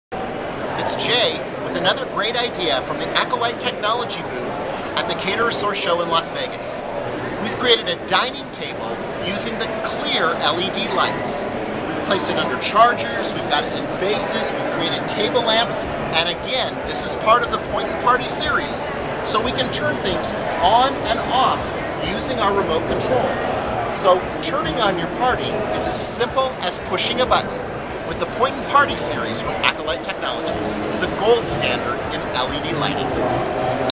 Point n Party Series Standard Remote Control Demo by Acolyte
The Point n Party Series by Acolyte makes it so much easier for event designers to "set up" their Acolyte illuminated decorations.  Set up your designs up to 2 days before the start of the event and then turn everything on with the click of a button.  Save time, money and a lot of headaches.  Watch this video as J. Schwanke demonstrates at the Acolyte Booth at the Catersource Show 2012.